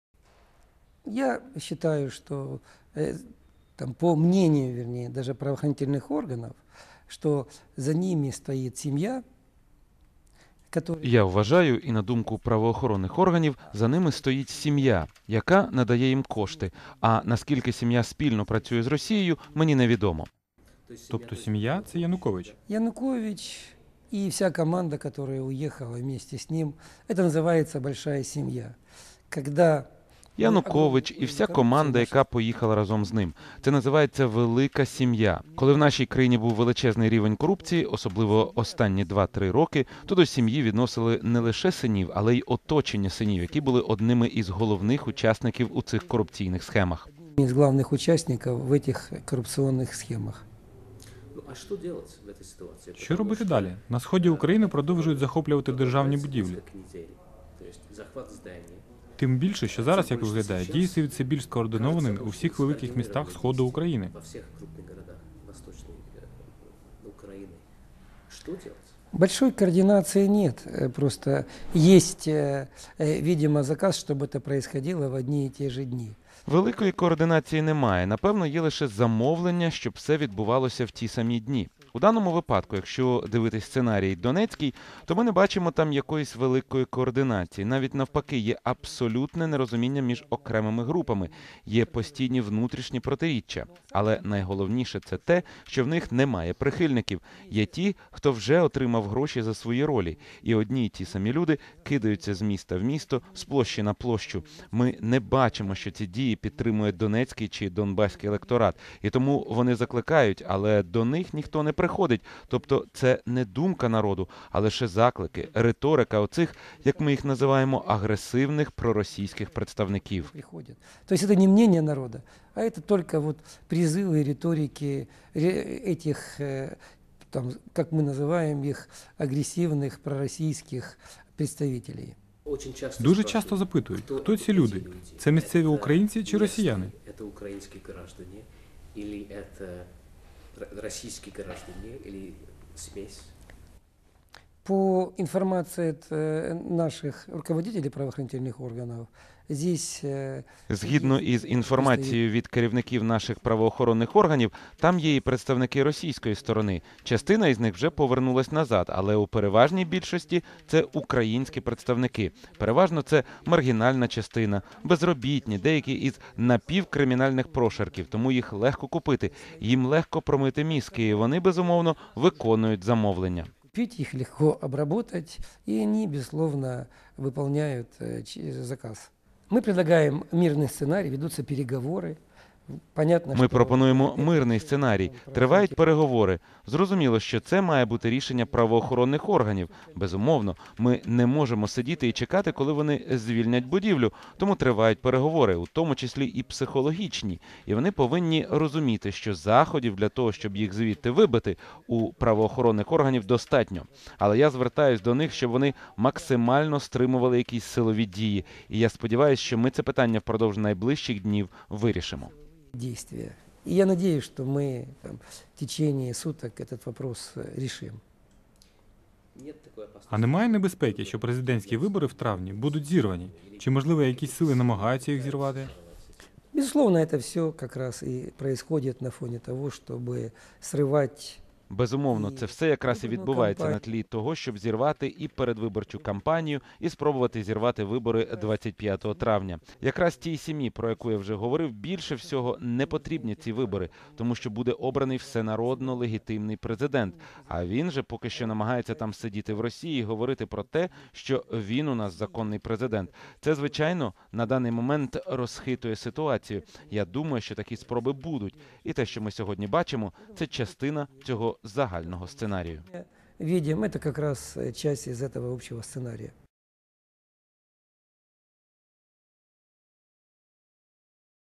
Тарута: Янукович хоче зірвати вибори
Губернатор Донецької області сказав, що за сепаратистами, які захоплюють будівлі облдержадміністрацій на сході України, стоять усунутий президент Янукович та його оточення.